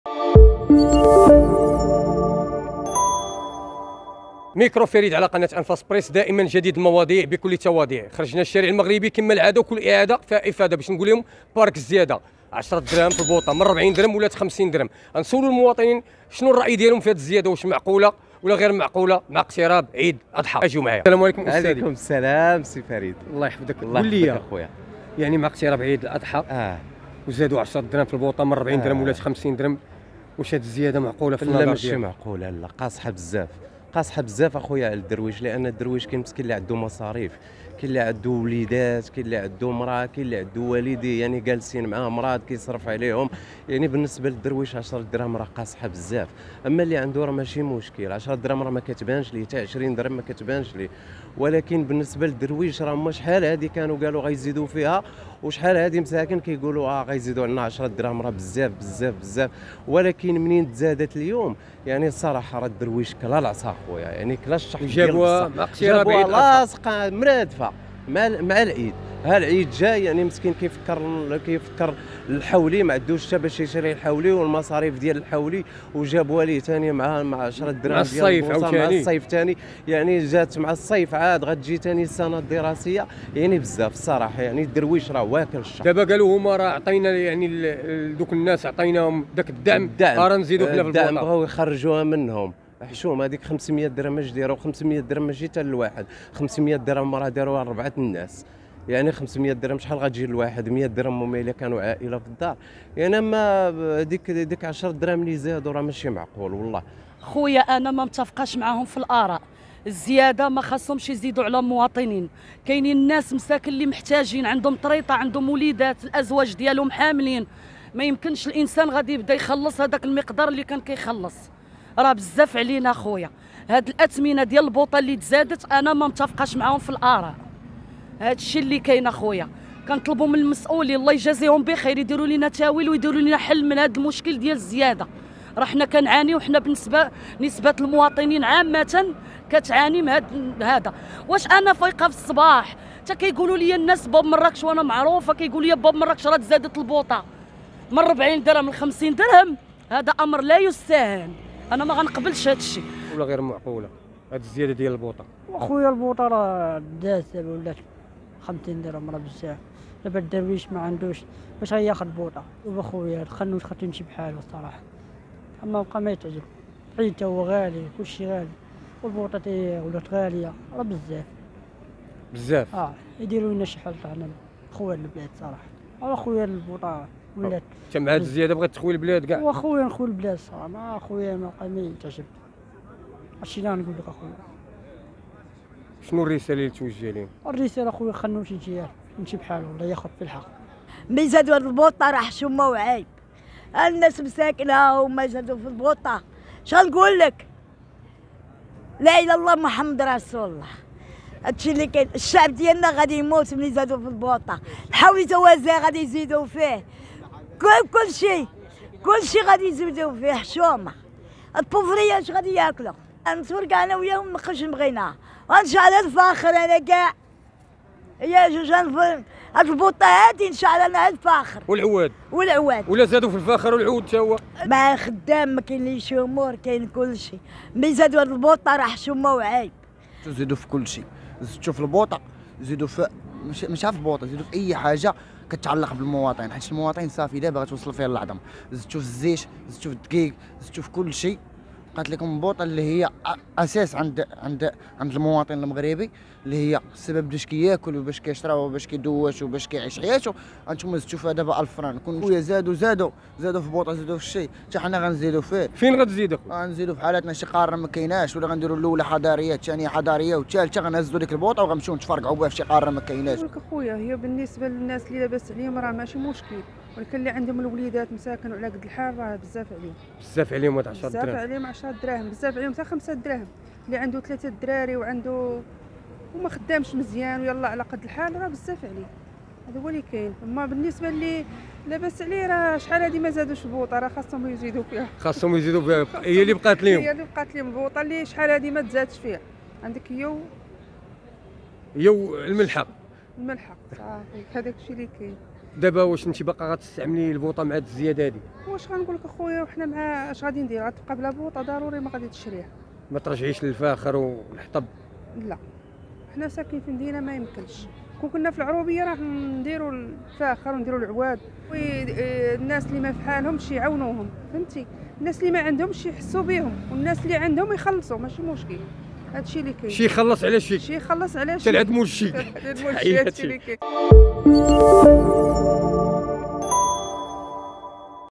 مغاربة: زيادة 10 دراهم في البوطا قاصحة بزاف وأخنوش خاصو يمشي بحالو
احتج مجموعة من المواطنين بسبب القرار الأخير للحكومة والقاضي بالزيادة في أسعار قنينات الغاز ، مؤكدين أن موجة الغلاء أصبحت تؤرقهم بشكل لا يطاق